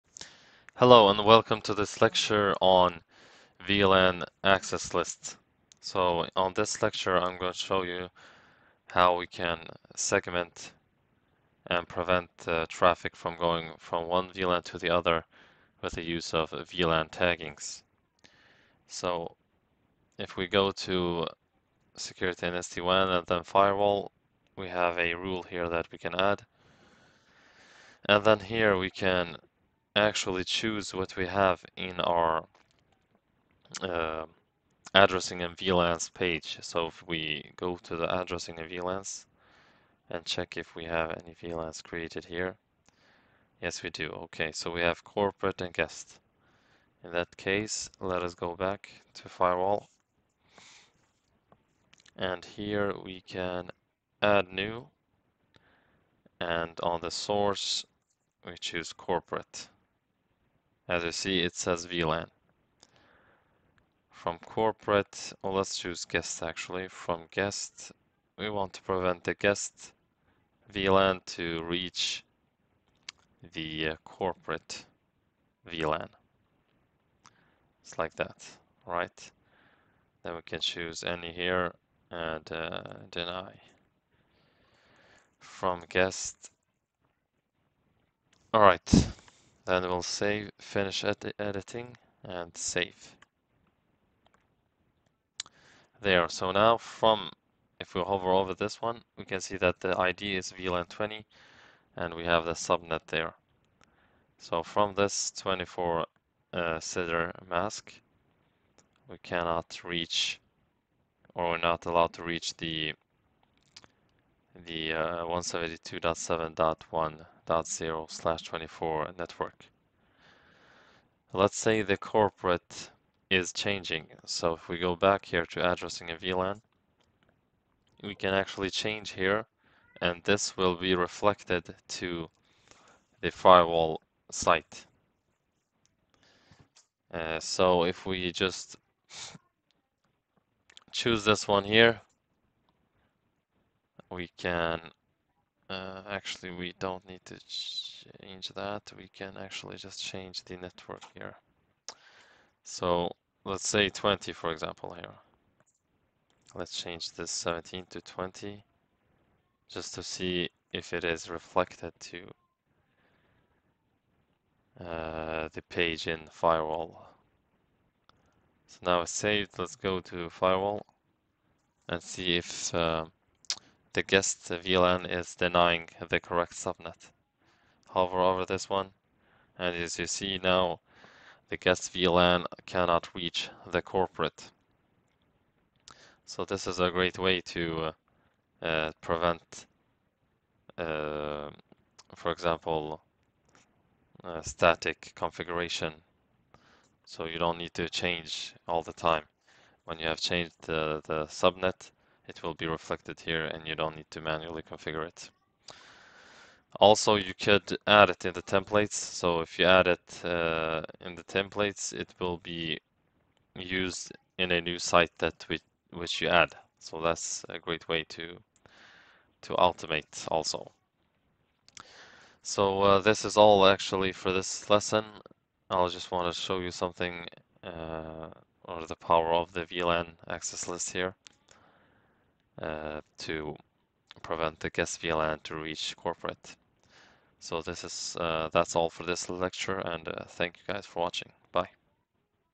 Meraki Vlan Access List
Welcome to this video, were I briefly explain the uses of VACL in Meraki MX and how it solves the problem of having to add manual acl.

In this case we want to protect corporate from guest network.